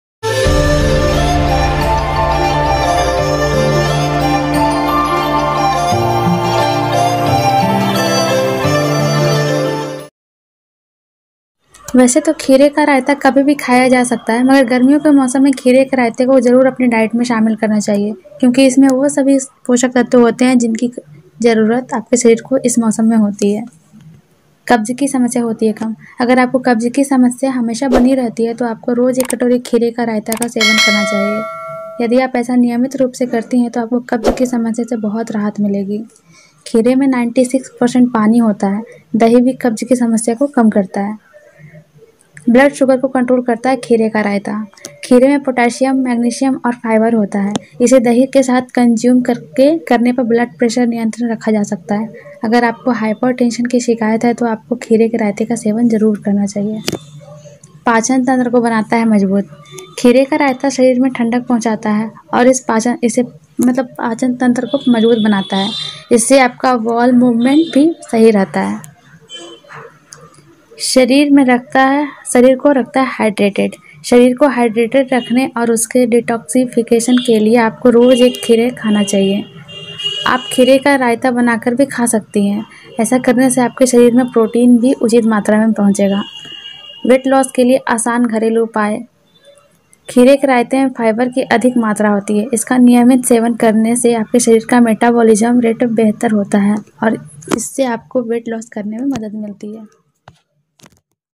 रोज खाएंगी 'खीरे का रायता' तो होंगे ये फायदे।।Benefits Of Cucumber Raayata।।Health Tips।।
वैसे तो खीरे का रायता कभी भी खाया जा सकता है, मगर गर्मियों के मौसम में खीरे के रायते को जरूर अपनी डाइट में शामिल करना चाहिए क्‍योंकि इसमें वह सभी पोषक तत्‍व होते हैं, जिसकी जरूरत आपके शरीर को इस मौसम में होती है।'
इस वीडियो को अंत तक जरूर देखें और ऐसी जानकारी पाने के लिए हमें सपोर्ट जरूर करें।